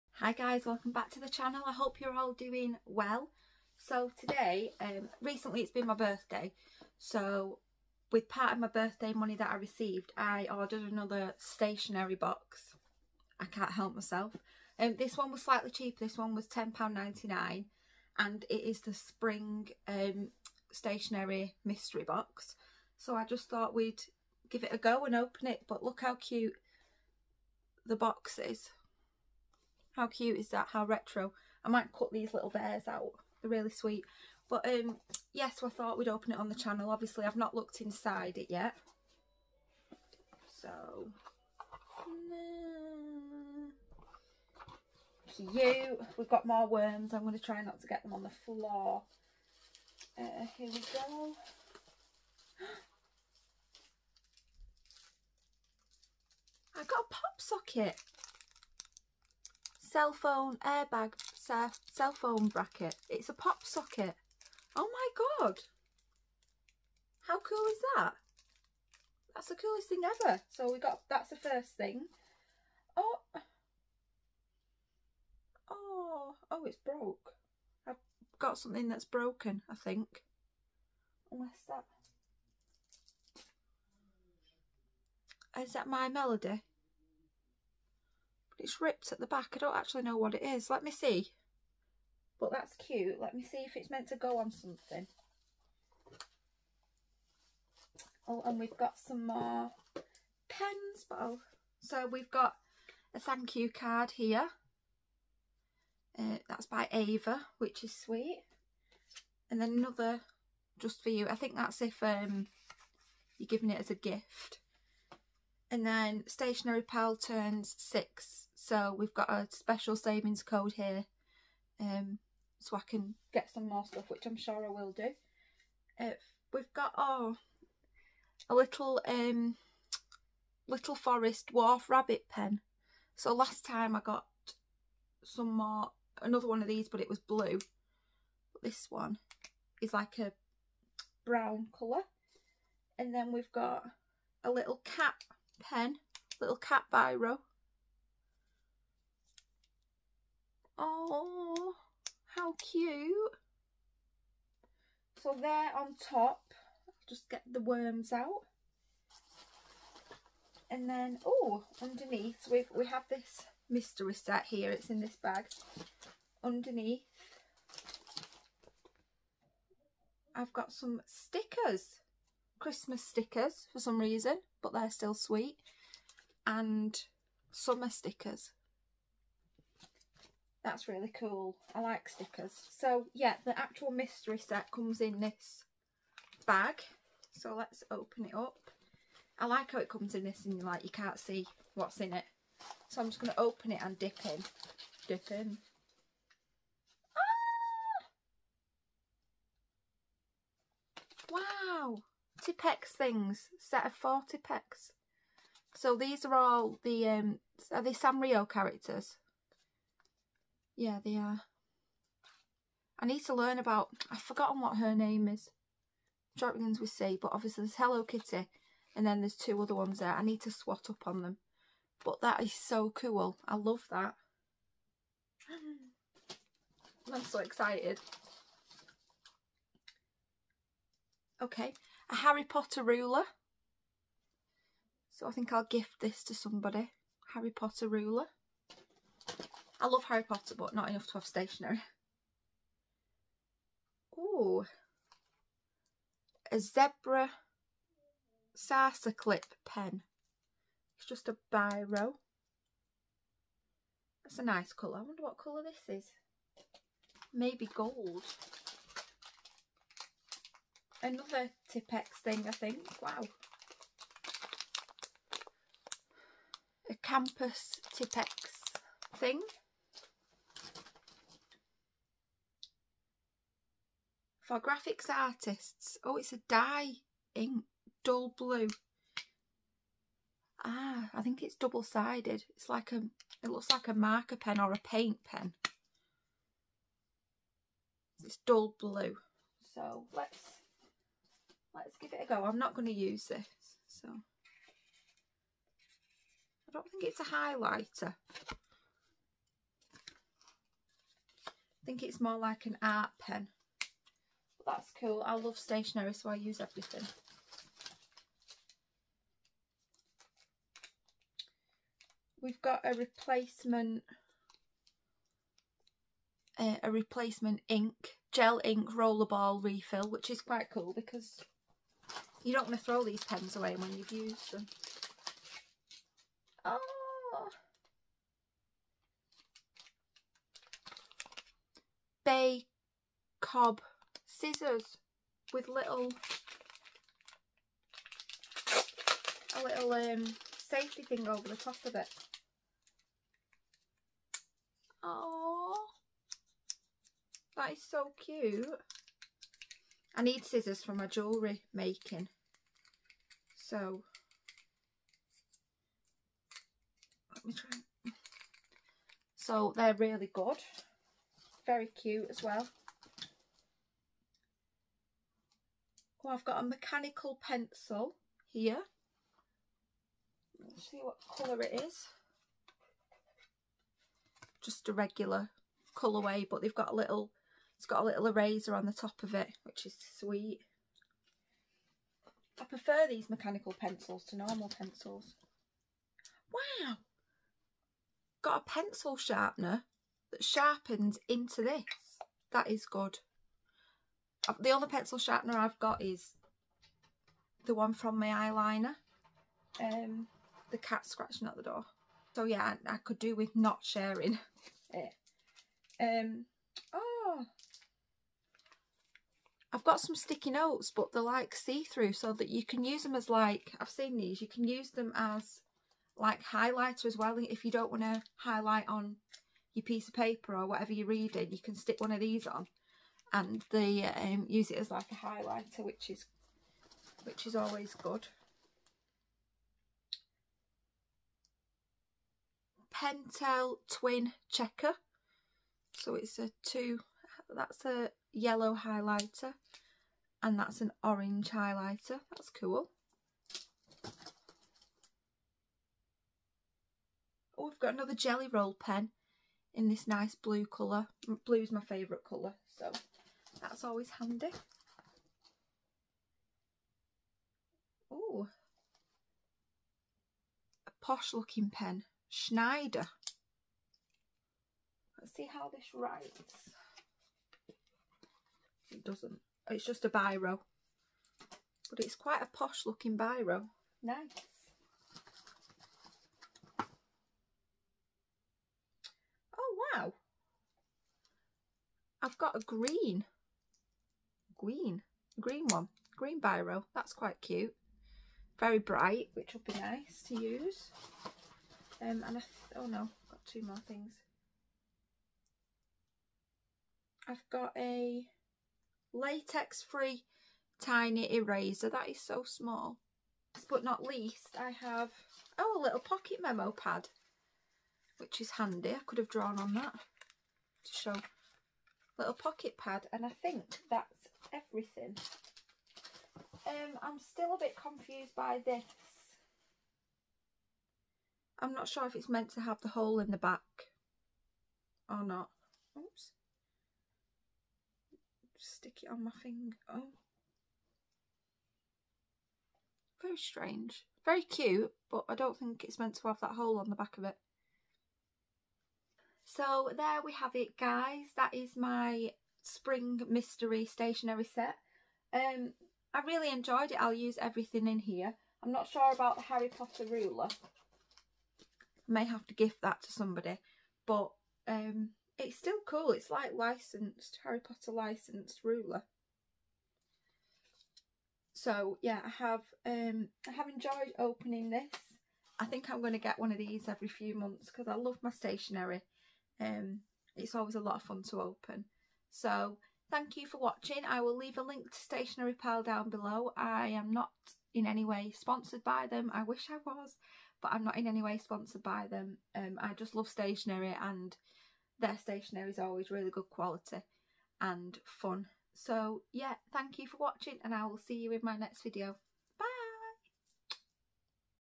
Spring Mystery Stationary Box Unboxing from Stationary Pal | Heather Louise x
Hi Guys!

Hope you enjoy this unboxing! I blooming love Stationary and love Stationary Pal!

Heather Louise x